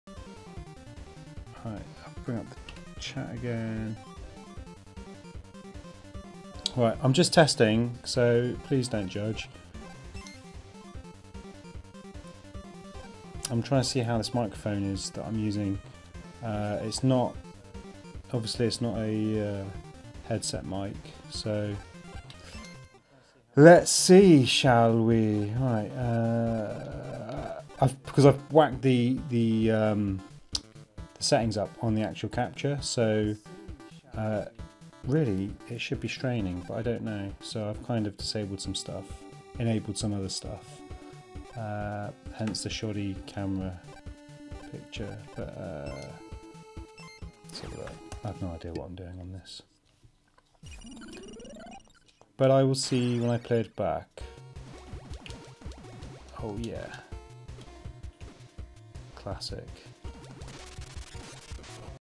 Samson Meteor Mic test via Twitch
Testing out the £60 Meteor Mic with Twitch.